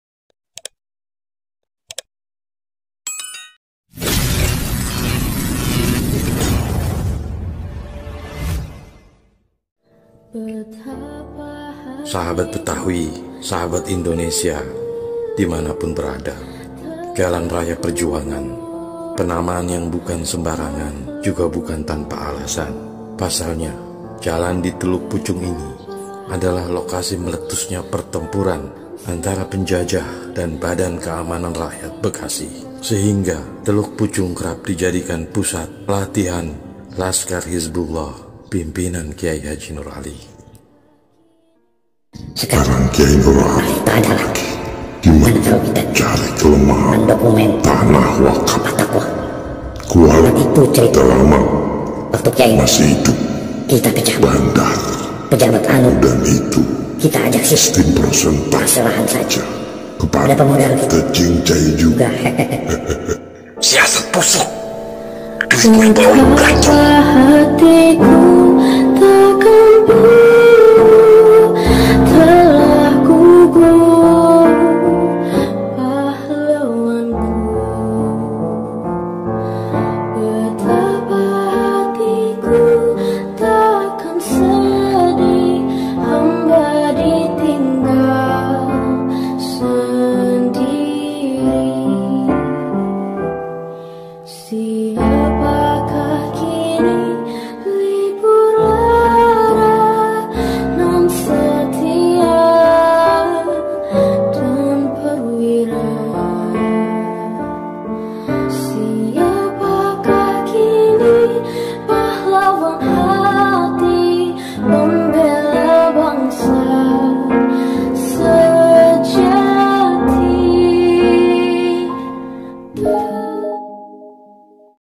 Mafia Tanah Tak Takut Tuhan
Mafia tanah yang hobi mengutak-atik tanah yang bukan haknya, khususnya tanah wakaf milik yayasan dan lembaga Islam, hidupnya akan menemukan kejumudan, kebuntuan dan gak tenang sepanjang hayat. Ditambah lagi dengan  penilaan masyarakat yang menganggap mafia tanah sebagai 'sampah sosial'